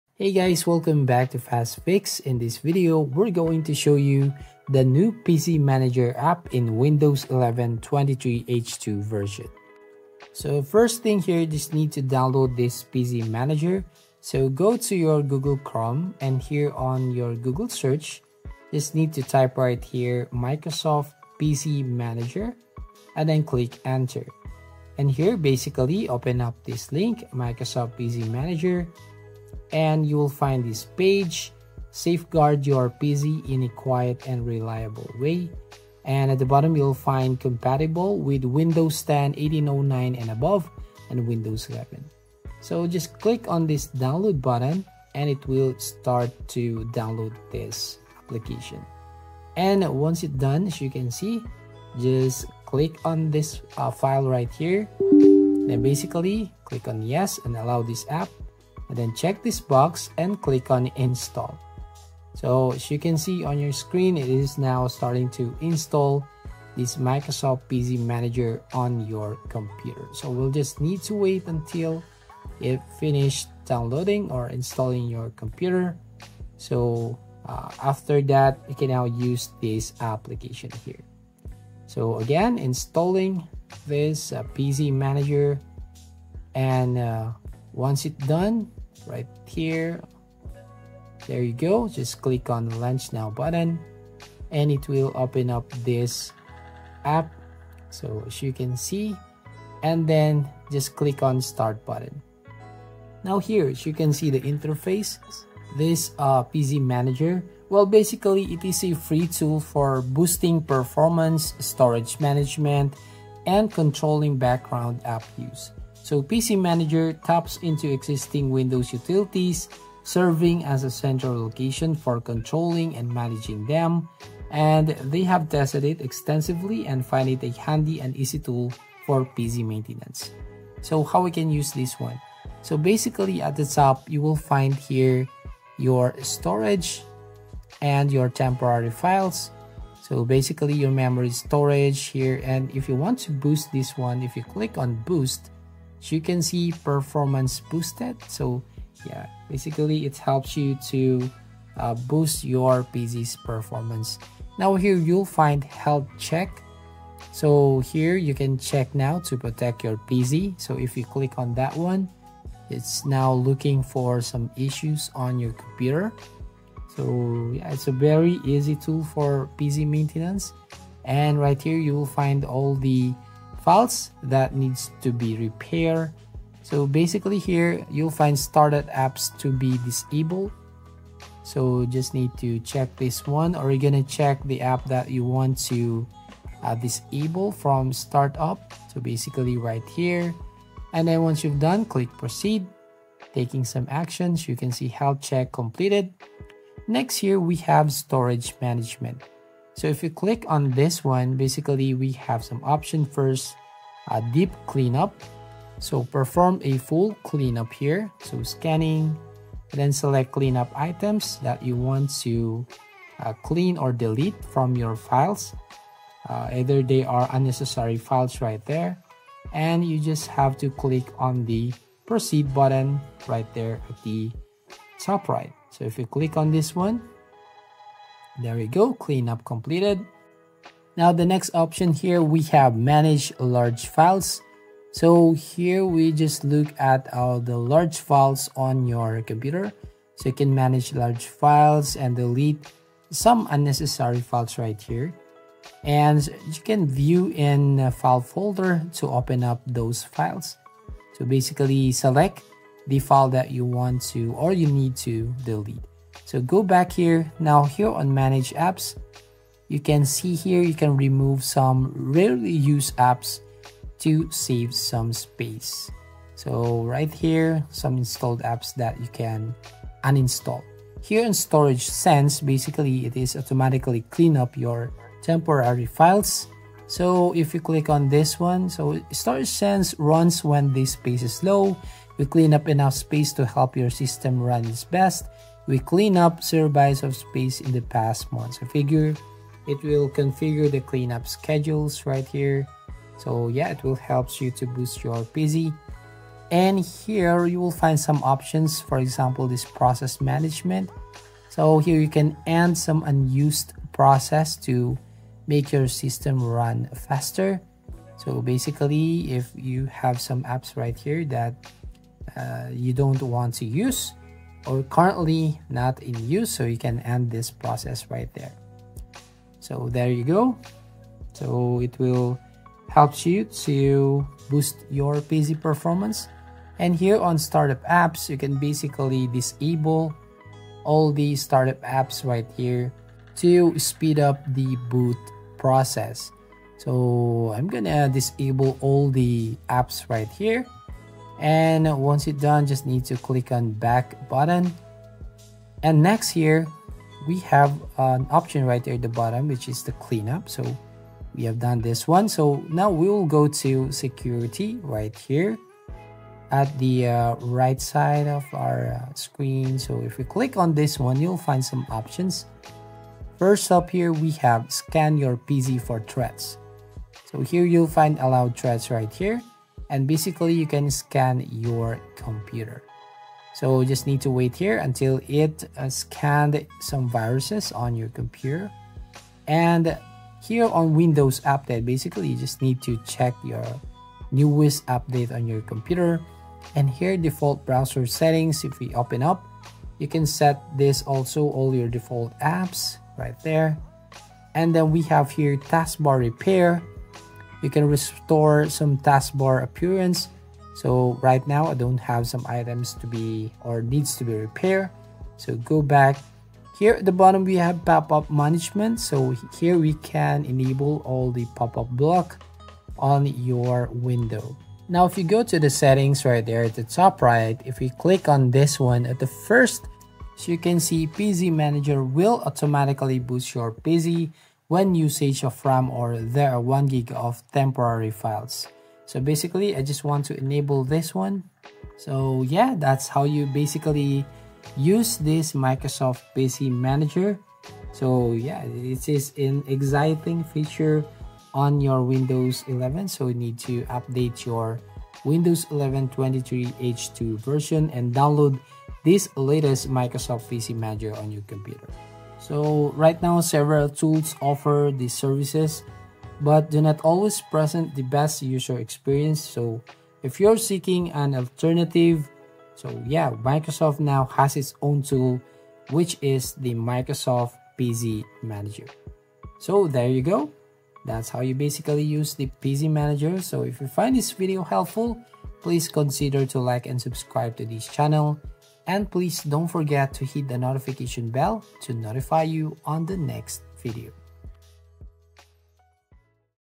I Tried The New PC Manager App in Windows 11 23H2!
The New Pc Manager App is easy to use and has a lot of nice features! Follow this video to the end to see all the new things you can do with the microsoft pc manager windows 11!